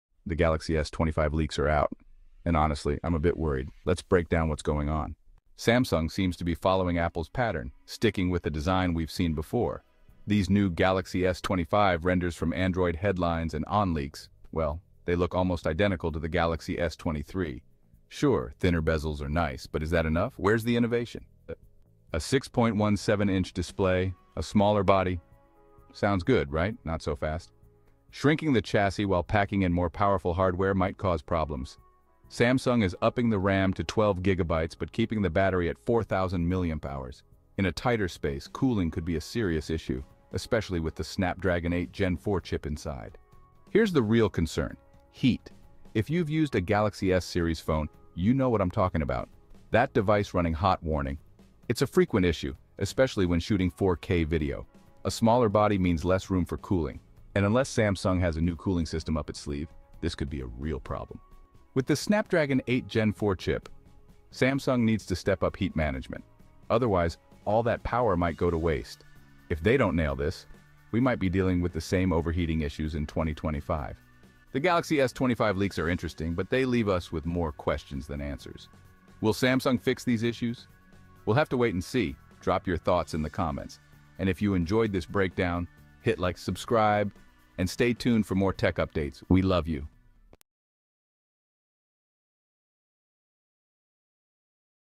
Samsung Galaxy S25 Leaks: Should We Be Worried?
The latest Samsung Galaxy S25 leaks have just dropped, and while the device looks sleek with its familiar design and thinner bezels, there's something to be concerned about. The leaks, provided by Android Headlines and @OnLeaks, show a design nearly identical to the Galaxy S23, with a 6.17-inch display and Snapdragon 8 Gen 4 processor under the hood. However, the smaller chassis and limited 4,000mAh battery raise concerns about potential overheating, a recurring issue with smaller Galaxy S models. With Samsung keeping the same design and battery capacity, will they be able to manage the heat and deliver a high-performance phone? In this video, we dive into the leaks and what they mean for the Galaxy S25’s performance, design, and user experience. Could this be a case of style over substance?